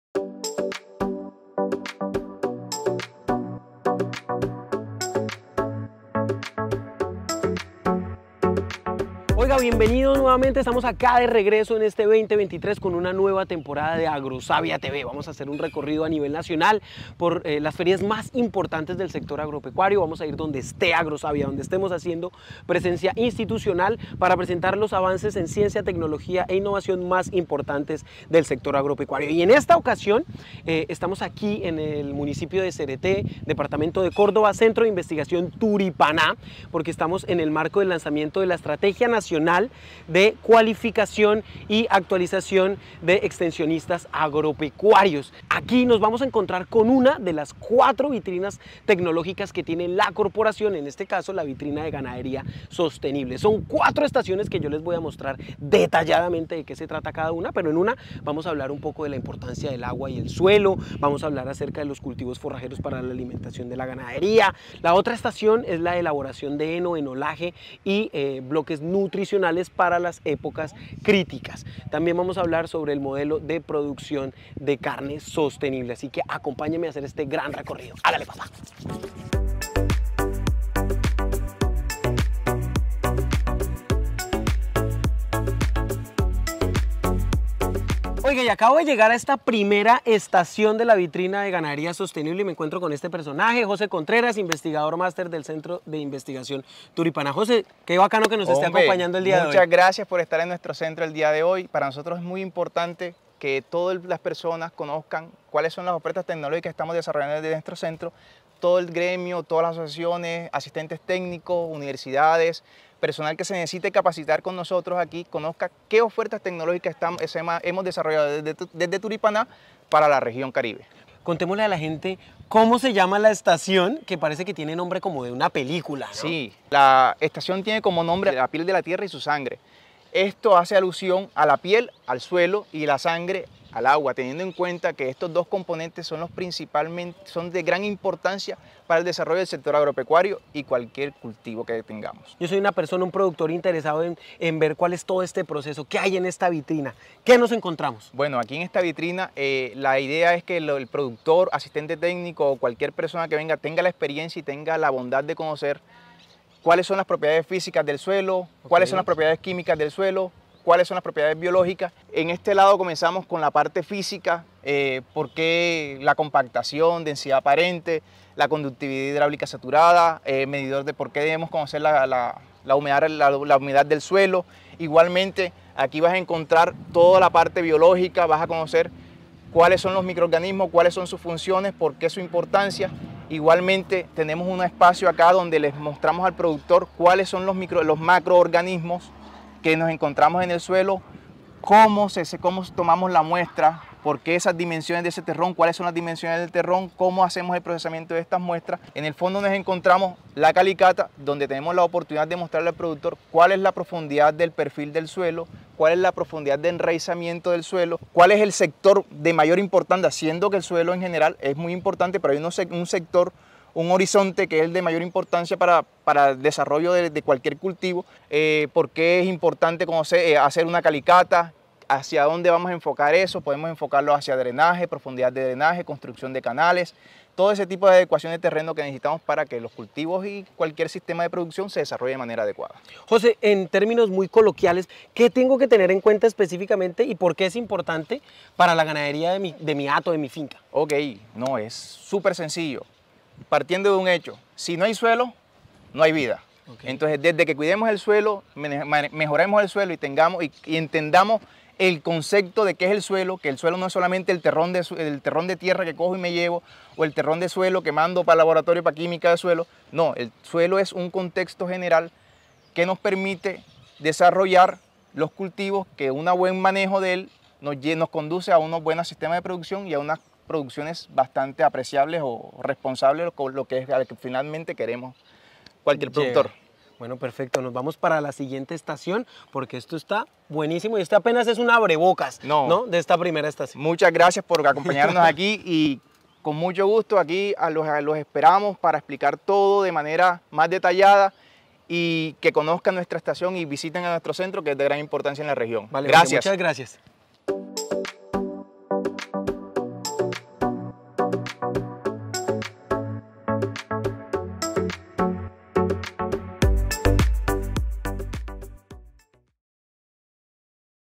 Vitrina Ganadería Sostenible CITuripaná - Módulo: La piel de la tierra
En el marco de la Estrategia Nacional de Cualificación y Actualización de Extensionistas Agropecuarios estuvimos en la vitrina de Ganadería Sostenible.

El módulo: La piel de la tierra, se enfoca en las temáticas del agua y el suelo son esenciales para aprovechar el potencial de la tierra, permitiendo que las especies, tanto de plantas como de animales, utilicen plenamente los demás factores de producción que elevan los rendimientos.